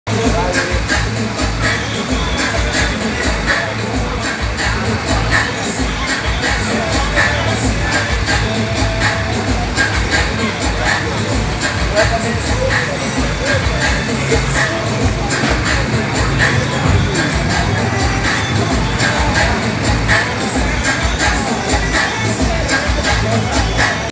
PASSINHOS DO CHAVES, CHIQUINHA, KIKO E SONIC DANÇANDO NA CARRETA DA ALEGRIA TRIPLO X, LIKE 62
SUPER PULO NINJA NA CARRETA DA ALEGRIA TRIPLO X, DANÇANDO FOFÃO, CHAVES E BEN10, LIKE (1)

1. Clique no link e baixe o Kwai.
2. Vincule meu código para receber R$1 imediatamente!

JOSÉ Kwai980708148